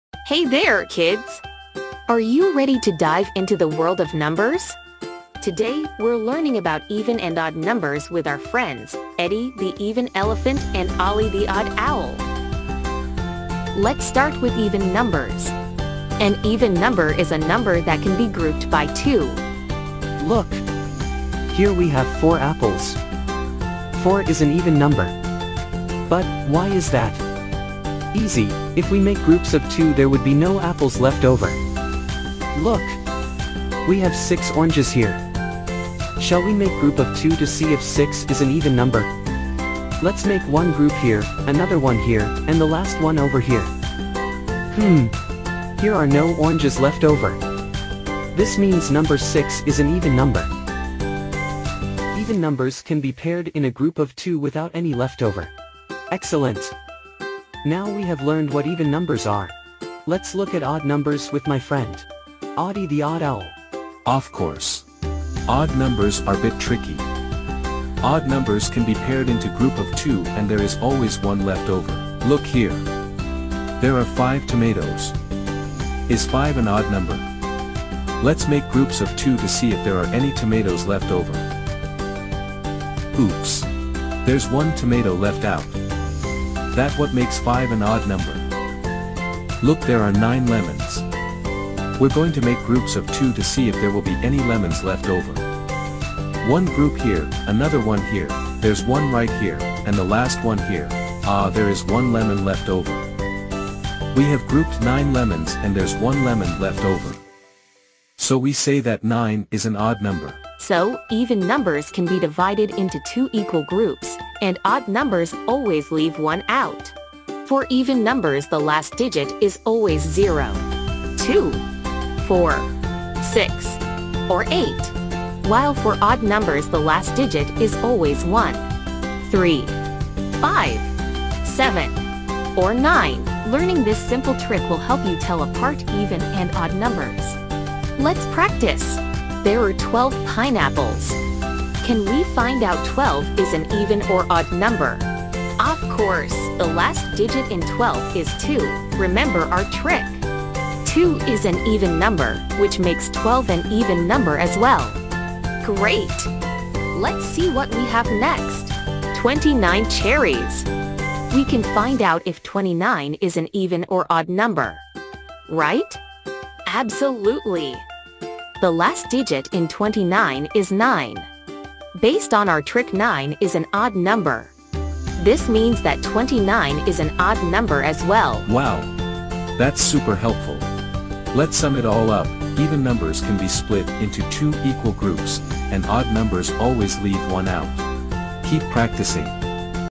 Even and Odd Numbers| Math for Kids
Join Eddie the Even Elephant and Olly the Odd Owl as they take you on a fun journey through the world of numbers! Learn how to tell even numbers from odd ones with playful examples and easy-to-understand visuals. Whether you're counting apples or splitting them into groups, Eddie and Olly are here to make learning numbers exciting! Perfect for kids and parents looking for a fun and educational adventure in math.